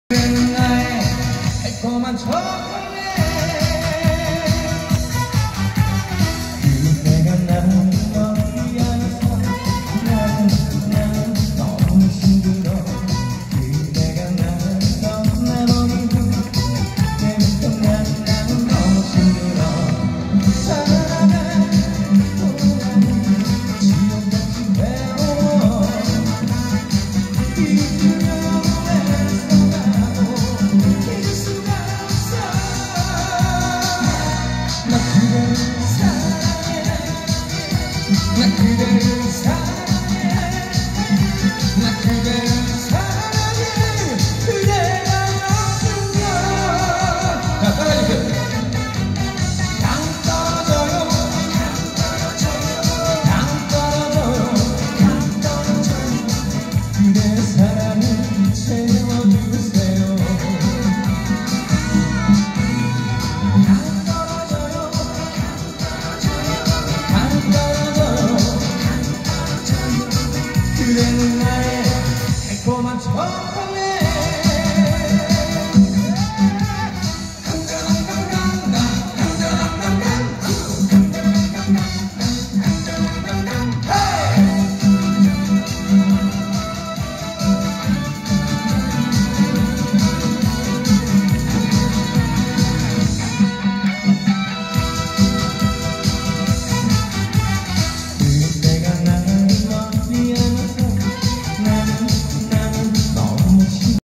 분당 야탑 행복콘서트 가수 성국
교통사고 없는 성남시를 위한 "행복콘서트"
한국교통안전공단, 성남시, 3개 경찰서가 만든 공익성 콘서트입니다.